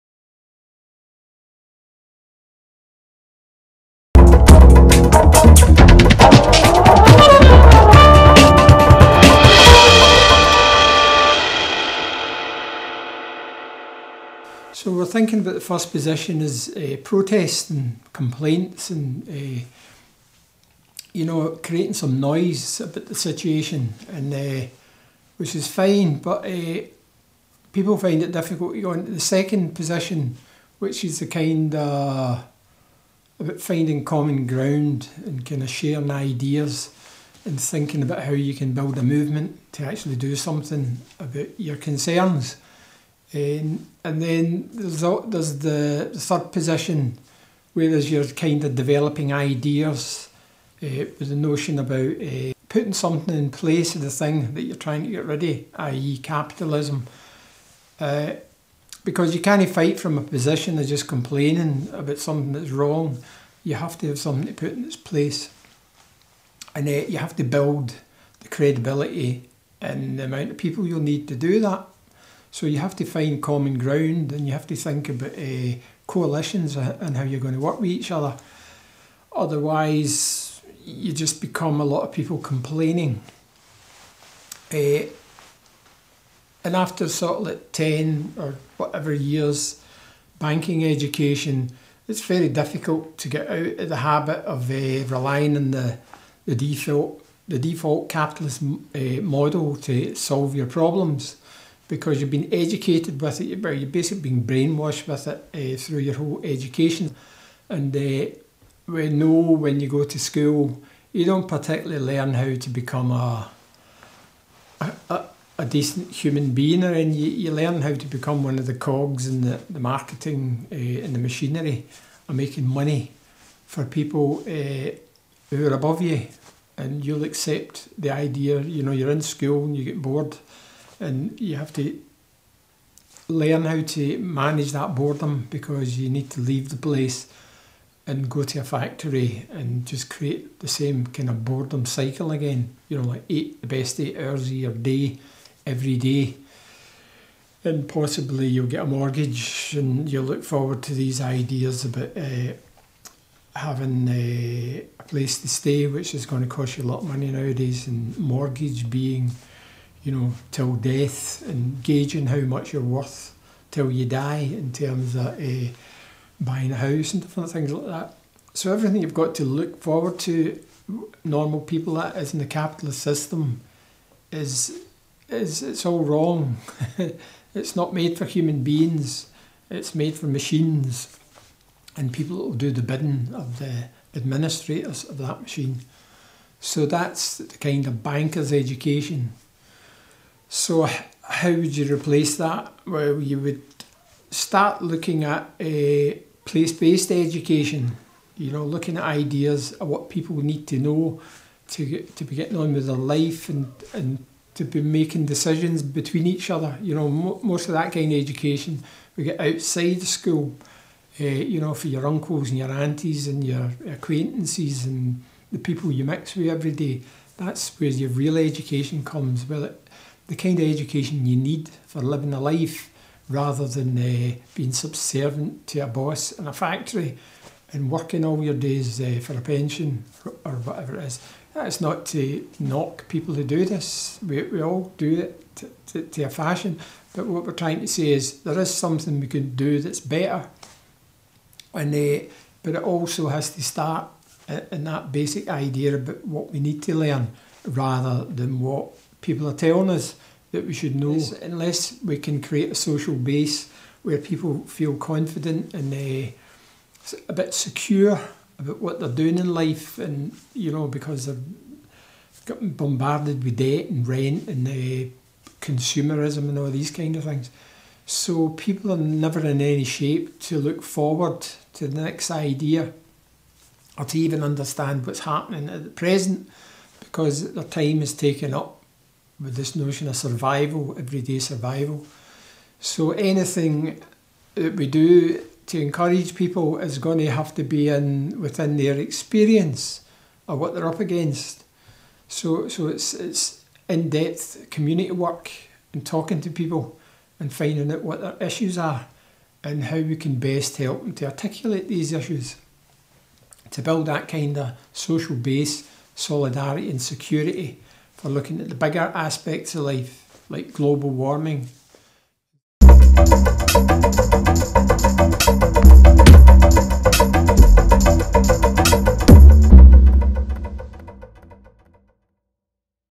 [TRIP Soundbites 1.] Banking education v place based learning
The Radical Imagination Project
"In a "banking" type of environment, a classroom is structured such that the primary duty of students is to remember and accurately recall the information provided by the instructor. They are not asked to participate in any other way, and simply absorb the information. In this type of approach, the world is seen as static and unchangeable, and students are simply supposed to fit into it as it is, as noted by Freire.

This concept is in contrast to the "problem solving" concept of education, in which students engage in dialogue about the subject matter with the teacher and also with one another. The world is seen as a work in progress, and students are encouraged to think about ways to change it."

radicalimagination.co.uk